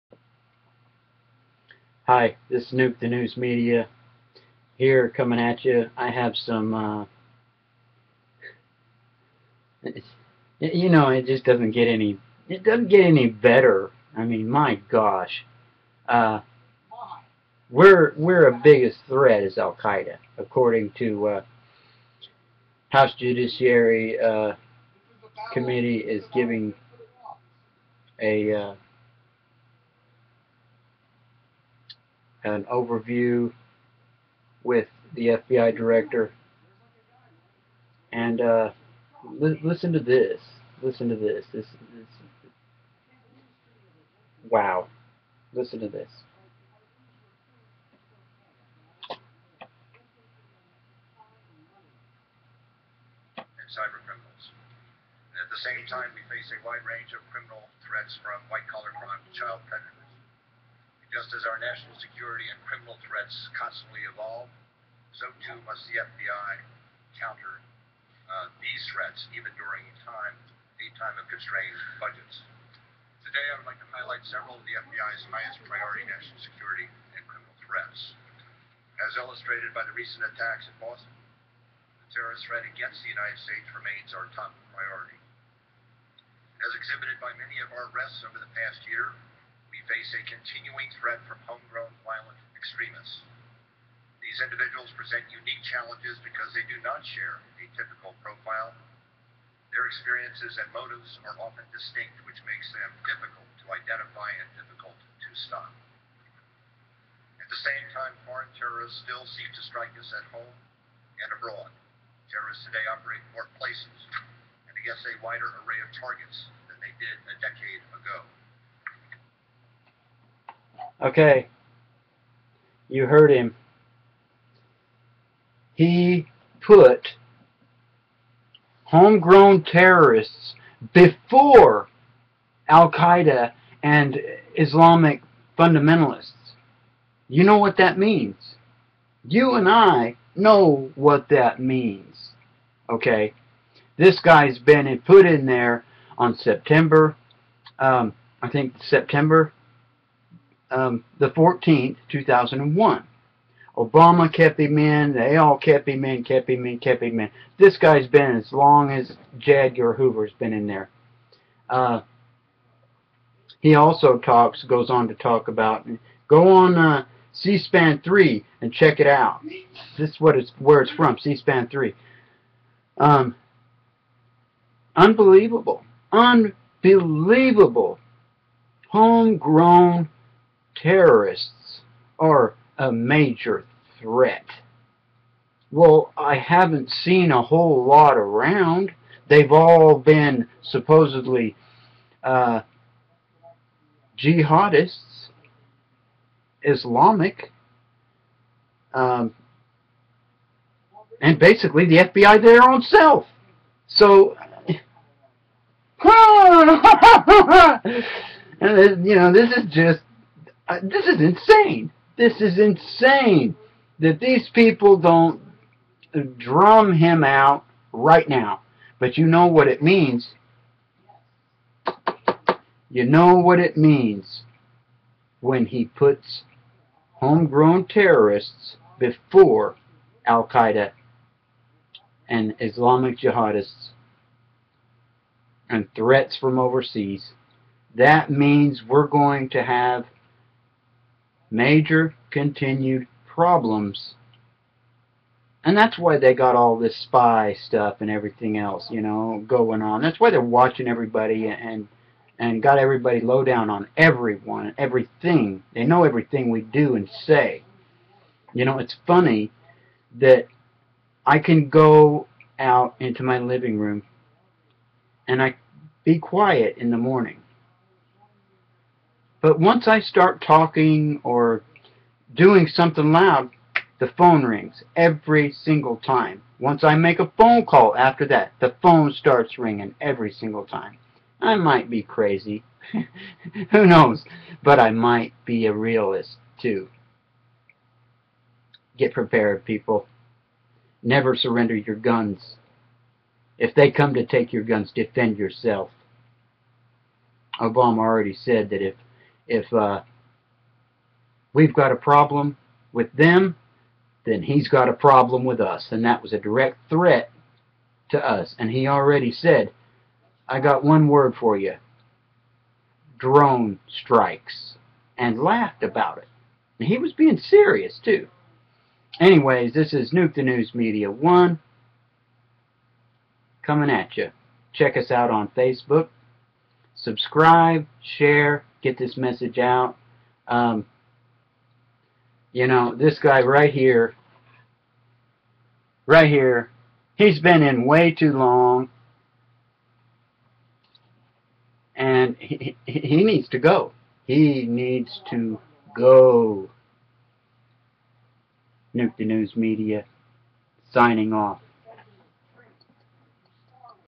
HOMEGROWN THREAT HIGHER ON FBI LIST OF DANGER THAN AICIDA/FBI DIRECTOR ROB MUELLER CSPAN 3
GET READY FOLKS FBI DIRECTOR MUELLER LISTS HOME GROWN TERRORIST HIGHER ON LIST THEN ALCIDA,FOREIGN JIHADIST!UNREAL.BE PREPARED.MUCH MORE STUFF THE SLEEPERS WILL NEVER HERE.SHARE.GO CHECK IT OUT ON C-SPAN.WHAT NEX?,SUBSCRIBE,LIKE,COMMENT.THANKS FROM NUKETHENEWSMEEDIA.CHECK US OUT ON F BOOK